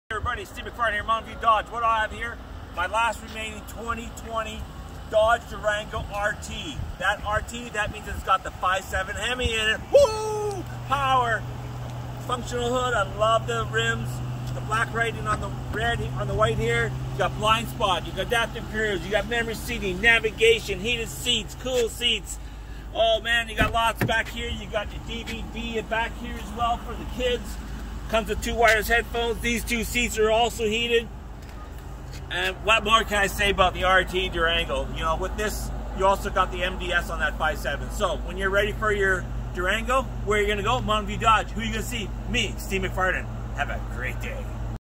2020 Dodge Durango R/T | STK # AL203 | Mountain View Dodge
Steve at Mountain View Dodge showing us his last New 2020 Dodge Durango R/T! This Durango comes with a 5.7L HEMI VVT V8 engine, Functional Hood, Heated and Cooled Seats, Nappa Leather-Faced Bucket Seats, Rear DVD Entertainment Centre and a Power Sunroof

STK # AL203
VIN # 1C4SDJCT7LC412864

32580 11 Range Road #43
Olds, AB
T4H 1P6